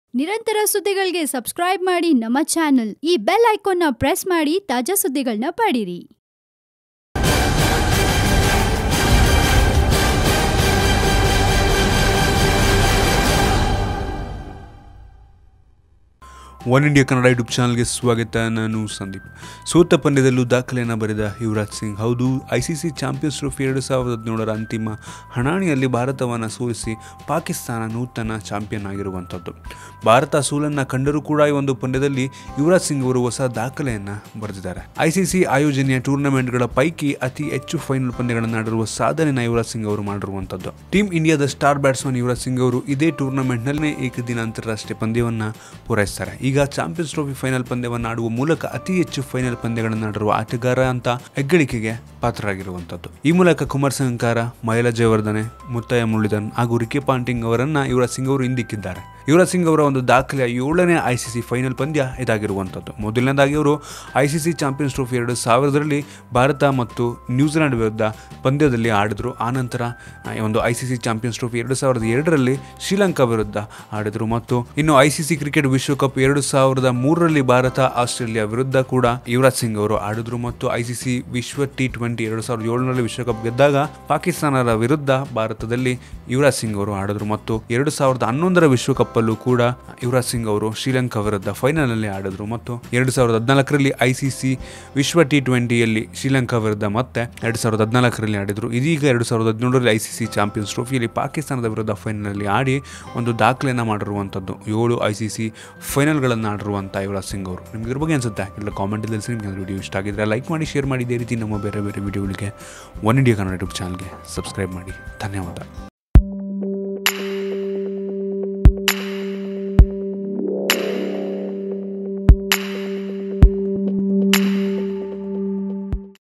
CT2017 :Yuvraj Singh Created A Unique Record Even After Big Loss | Oneindia Kannada
Ace Indian batsman Yuvraj Singh created a unique record on Sunday (June 18) by appearing in the ICC Champions Trophy 2017 final against Pakistan.

ಐಸಿಸಿ ಚಾಂಪಿಯನ್ಸ್ ಟ್ರೋಫಿ 2017ರ ಅಂತಿಮ ಹಣಾಹಣಿಯಲ್ಲಿ ಭಾರತವನ್ನು ಸೋಲಿಸಿ, ಪಾಕಿಸ್ತಾನ ನೂತನ ಚಾಂಪಿಯನ್ ಆಗಿದೆ. ಭಾರತ ಸೋತರೂ ಈ ಪಂದ್ಯದಲ್ಲಿ ಯುವರಾಜ್ ಸಿಂಗ್ ಅವರು ಹೊಸ ದಾಖಲೆ ಬರೆದಿದ್ದಾರೆ , ಈ ವಿಡಿಯೋ ನೋಡಿ
Oneindia kannada